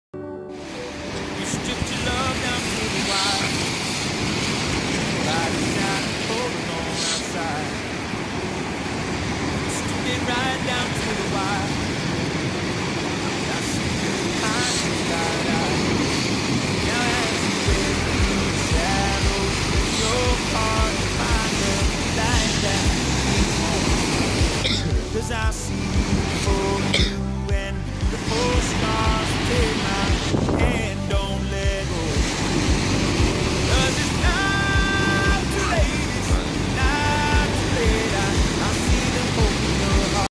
La ciotat - Grosses vagues
De belles vagues qui datent de l'hiver 2016. Snapchat : arnaud1344